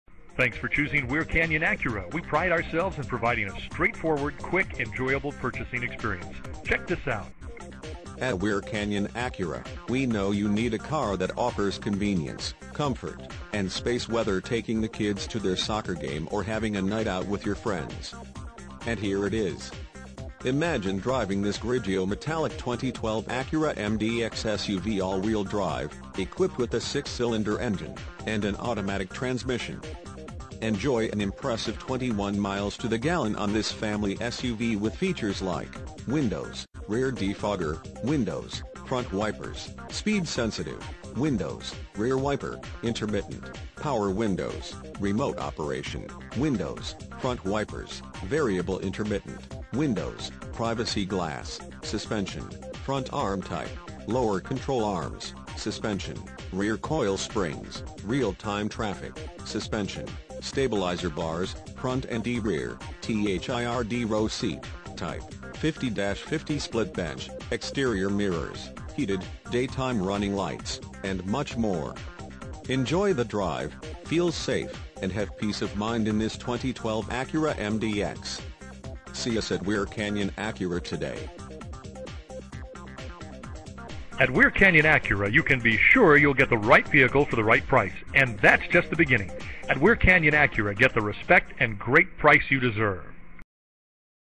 2012 Acura MDX Weir Canyon Acura Anaheim, CA 92807
Acura MDX At Weir Canyon Acura, we know you are looking for a vehicle to solve everyday task. Whether loading the kids soccer equipment or having a night out with your friends. You will have the room you need making those everyday task an ease to accomplish. Check out this Grigio Metallic Acura Mdx SUV AWD, equipped with a 6 Cylinder engine and an automatic transmission.
MILES: 38,000
VIN:2HNYD2H33CH533942

8323 E La Palma
Anaheim, California 92807